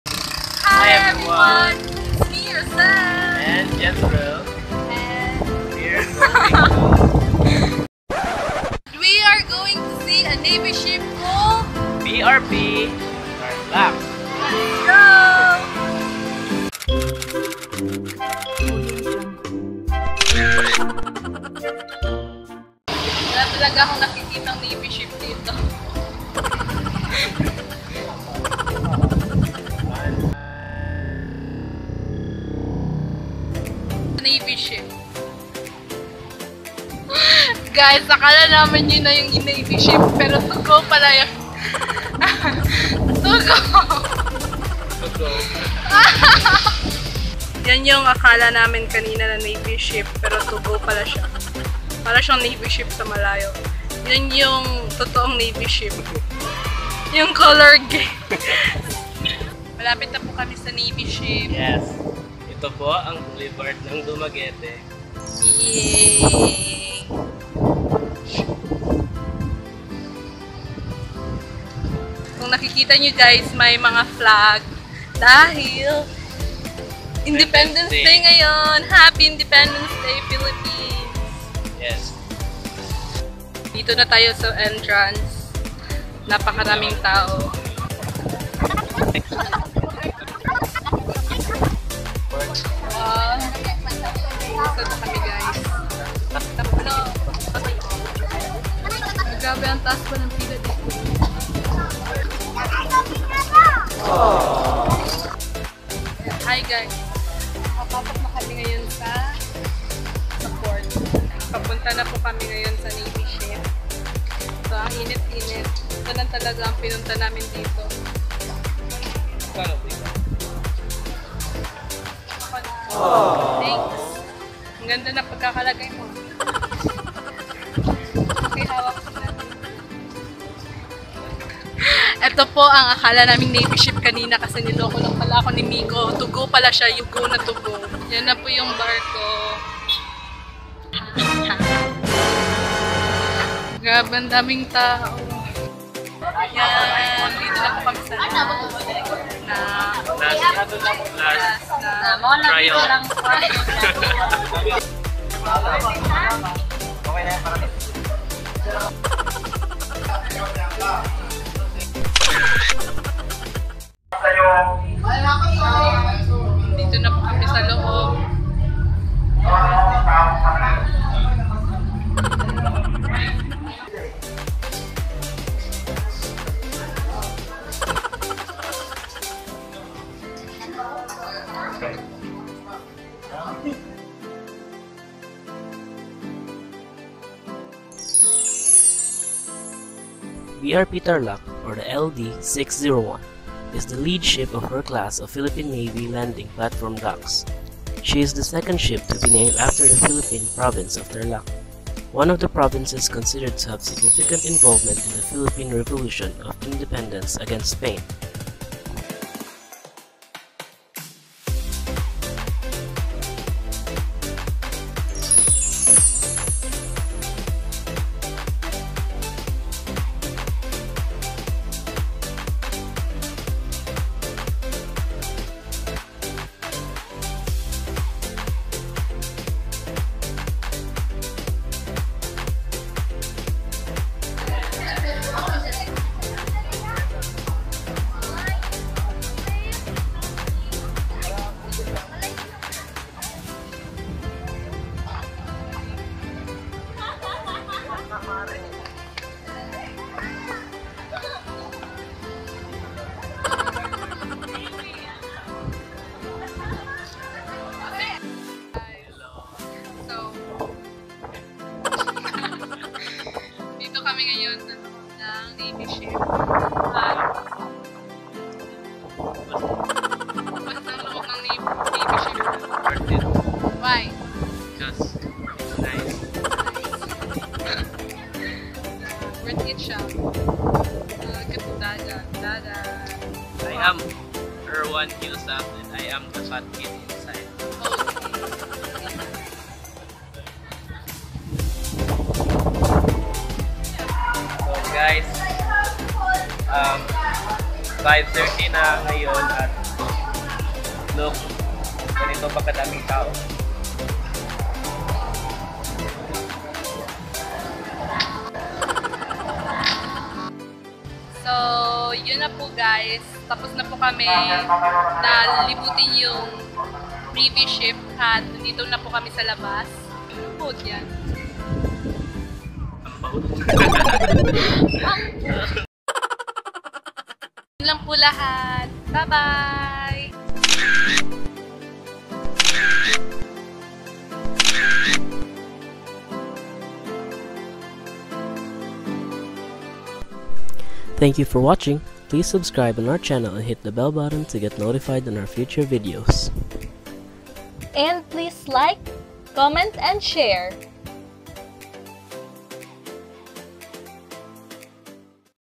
VISITING the BRP TARLAC (LD-601) in DUMAGUETE WOW! | Rocelle & Jezrelle
Hi everyone! For those of you who missed visiting the BRP Tarlac Navy Ship, don't worry because we got you covered. 😂😂